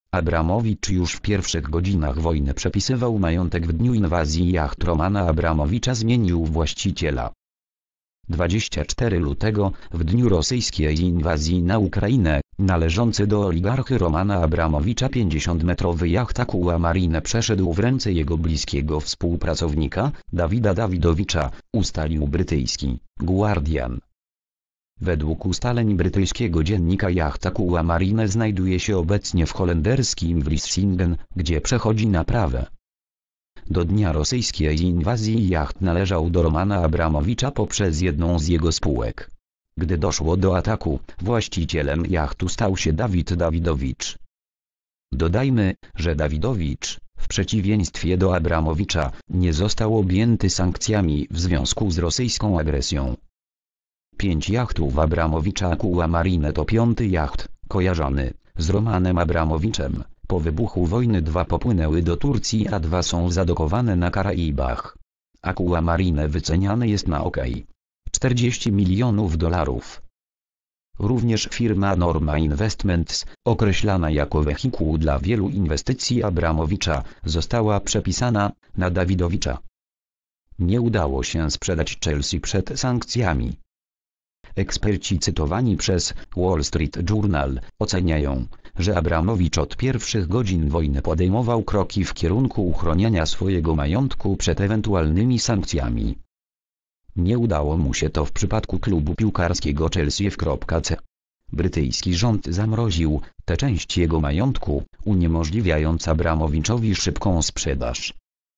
Abramowicz już w pierwszych godzinach wojny przepisywał majątek.
W dniu inwazji jacht Romana Abramowicza zmienił właściciela.

 (fot. Paul Gilham/Getty Images)